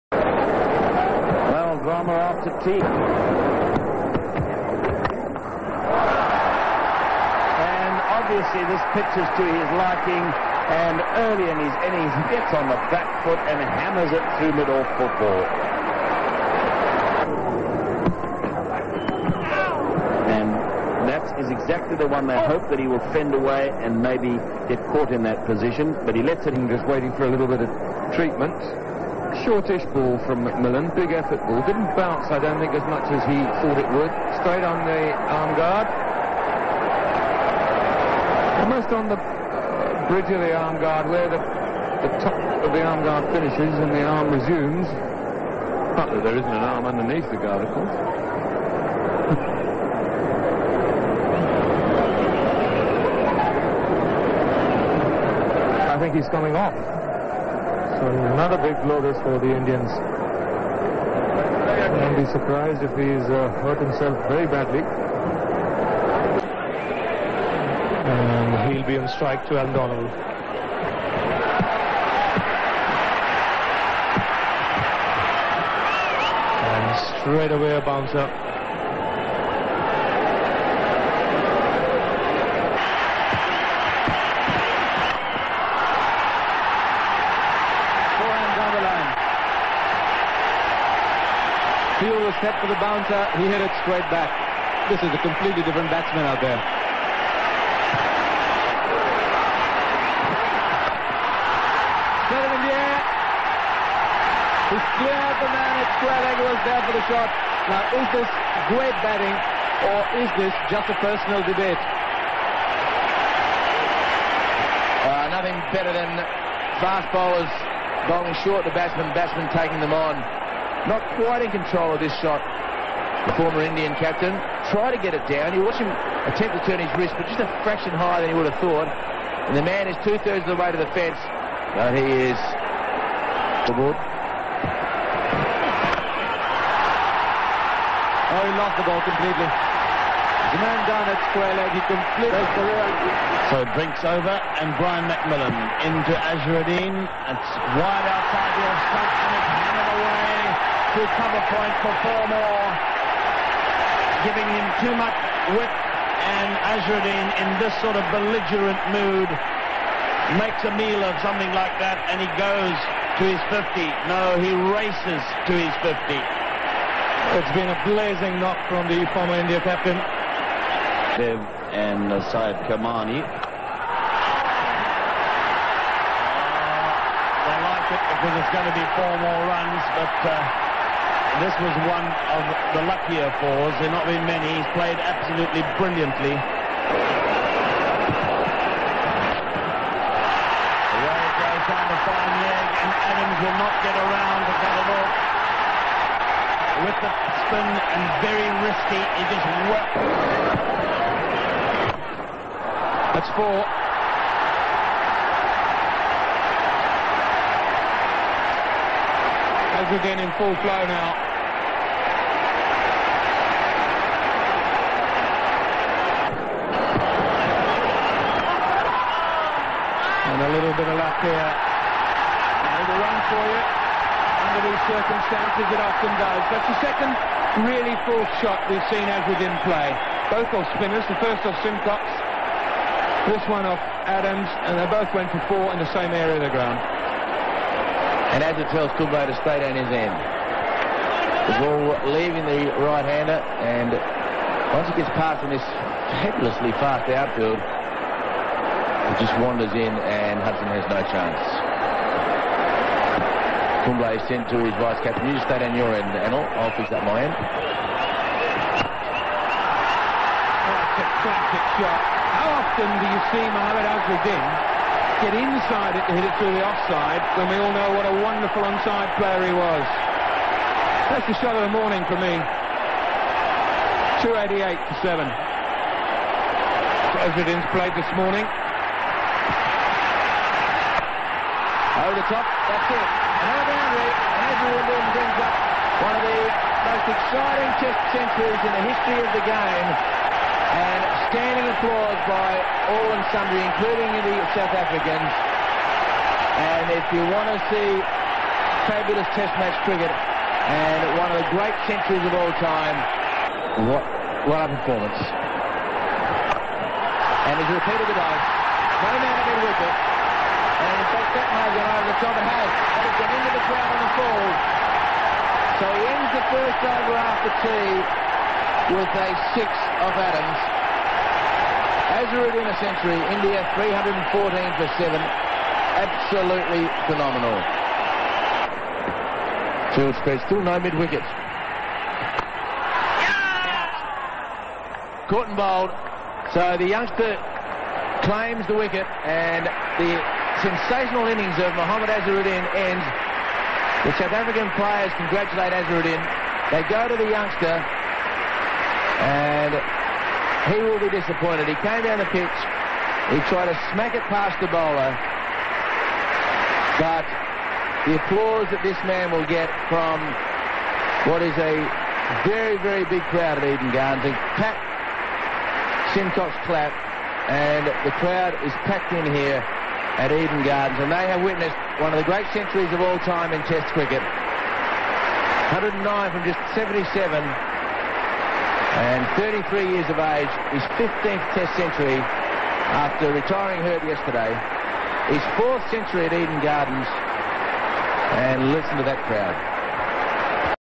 Mohammad Azharuddin 109 (77) vs South Africa (2nd Test) at Kolkata 1996
Courtesy : ESPN
Rare Footage of Azhar 15th Test Century - Magnificent Batting - One of the Fastest TEST Century -  Blazing Inning against South African bowling attack.
He hit 18 Fours & 1 Six.
Came to bat at 77/3 ,retired hurt at 87 came again to bat at 161/7 & then played one of the phenomenal test match century.
Anil Kumble 88 carrier best.